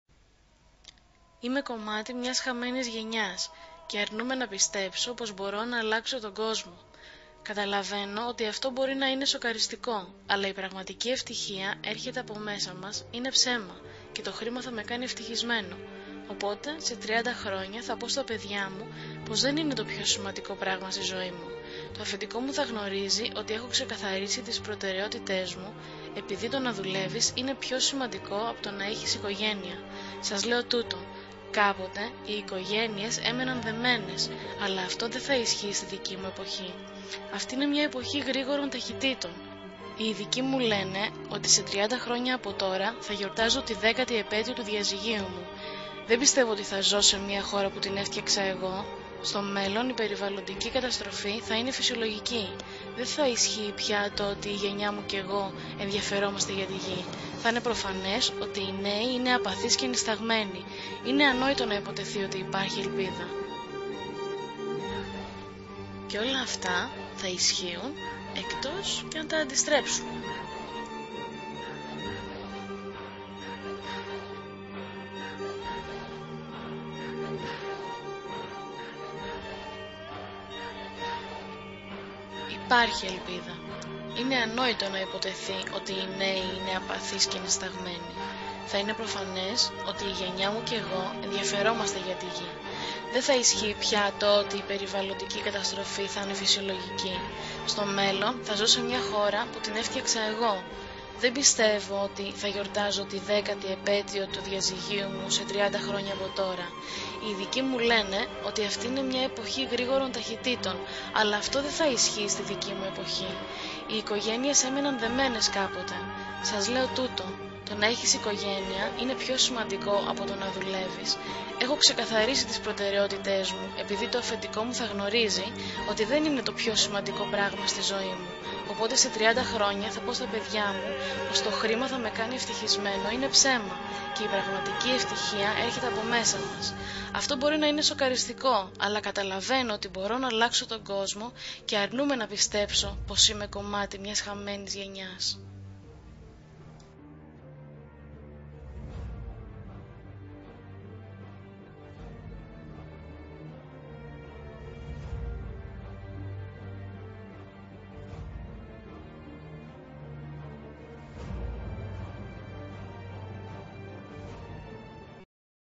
Χαμένη Γενιά...;
Μια γενιά που θεωρείται χαμένη...;
Μόνο αυτή η ίδια μπορεί να αντιστρέψει την πραγματικότητα που την αναγκάζει να χαθεί...

Το αυθεντικό κείμενο είναι του Jonathan Reed